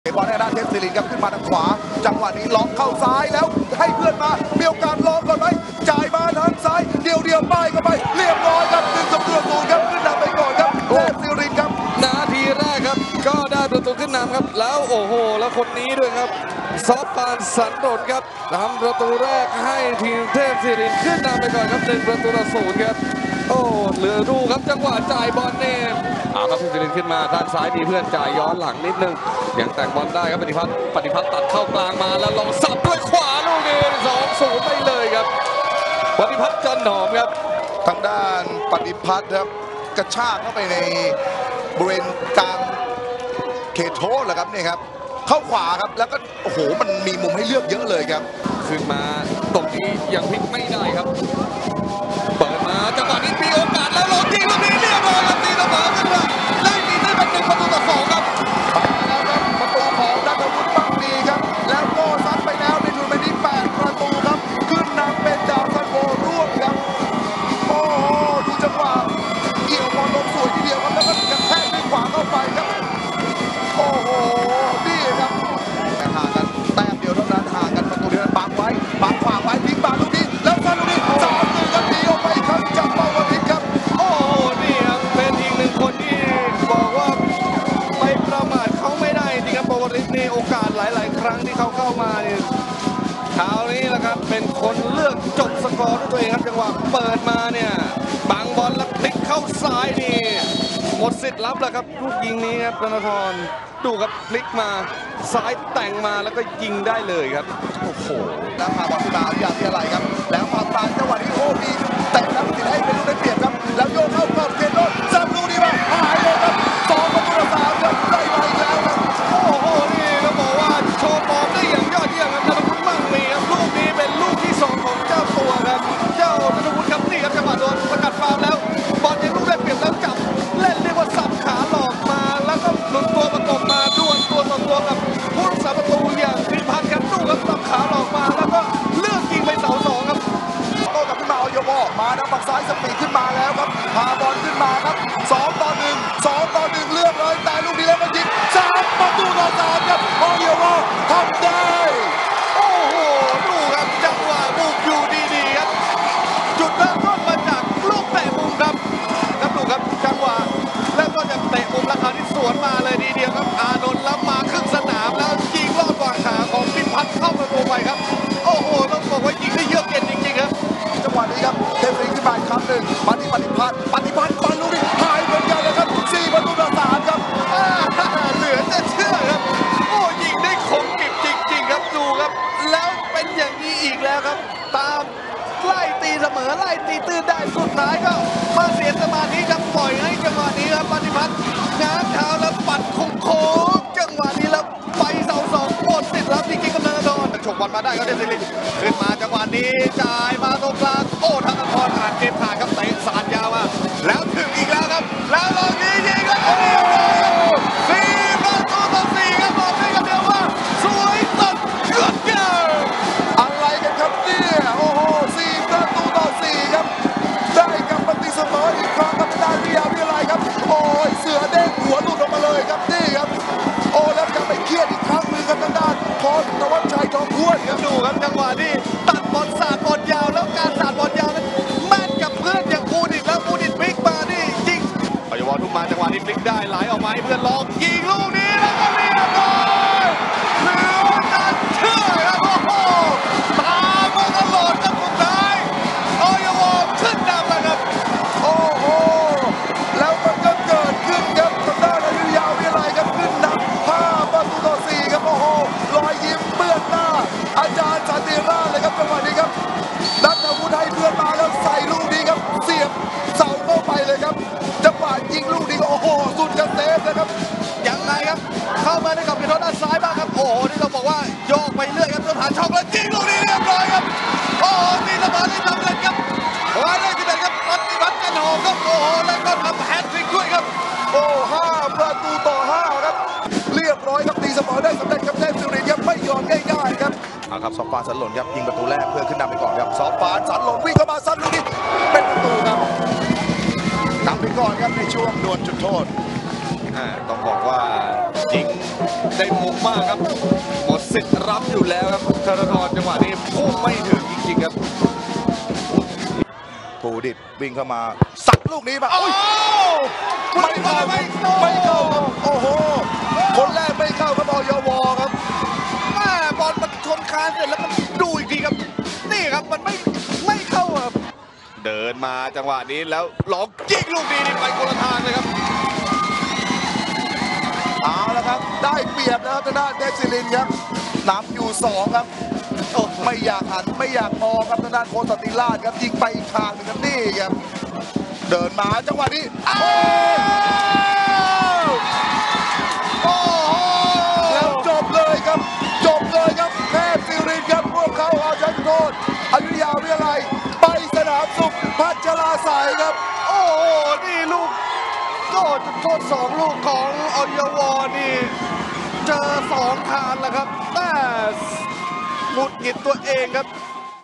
อยุธยาวิทยาลัย พบ เทพศิรินทร์ | ไฮไลต์รอบ 16 ทีม | ฟุตบอลแชมป์กีฬา 7HD 2024
ไฮไลต์รอบ 16 ทีม
อยุธยาวิทยาลัย (0)5-5(2) เทพศิรินทร์
ติดตามโปรแกรมการแข่งขัน ตารางถ่ายทอดสด และคลิปย้อนหลังทุกคู่ ได้ที่